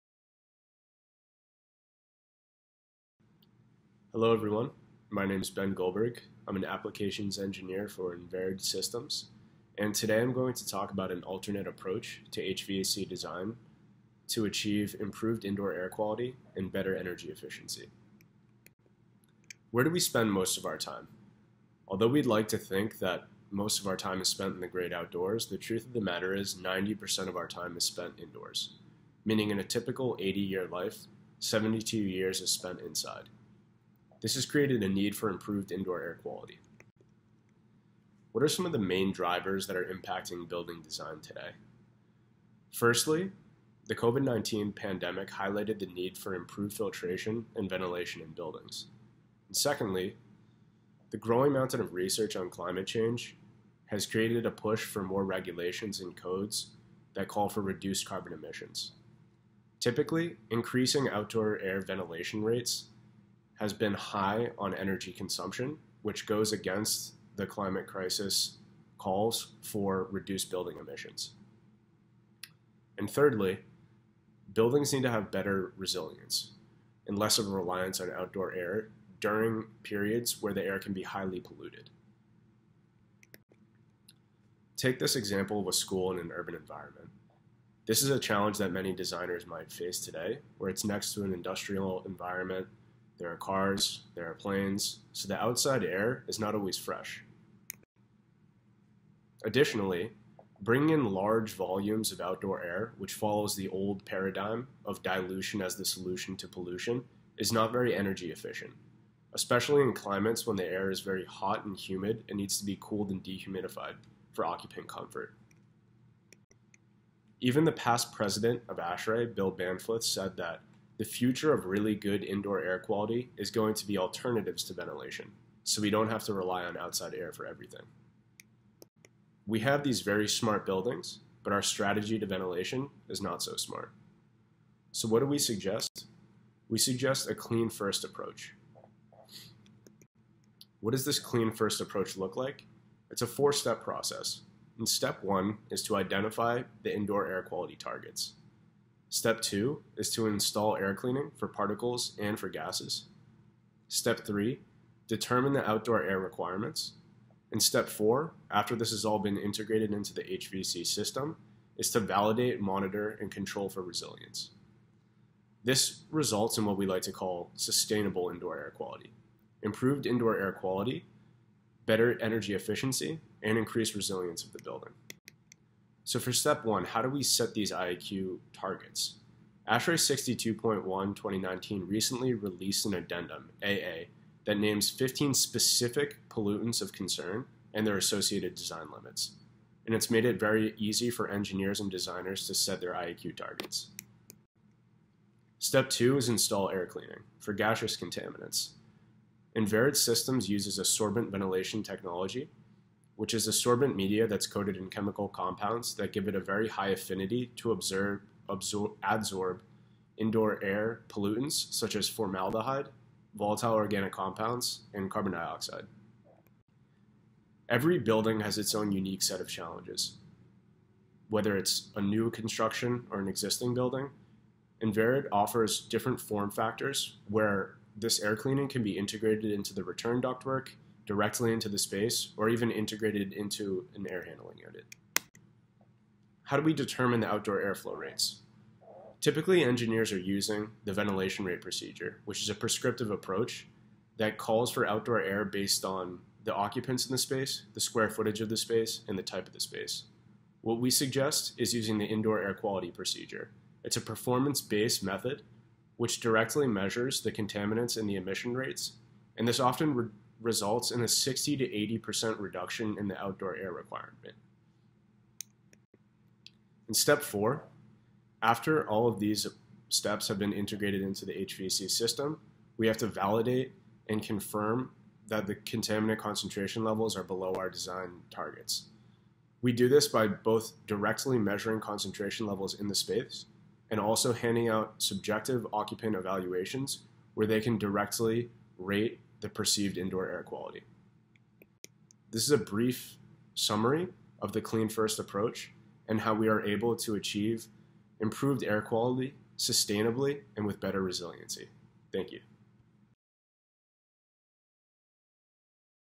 Reshaping HVAC Design: Designing for Improved Indoor Air Quality & Energy Efficiency
Ben Goldberg | enVerid Systems

An overview of the trends impacting building HVAC Design today, and how we can employ a Clean First Approach to improve indoor air quality in buildings while also reducing energy consumption and carbon emissions to achieve Sustainable Indoor Air Quality.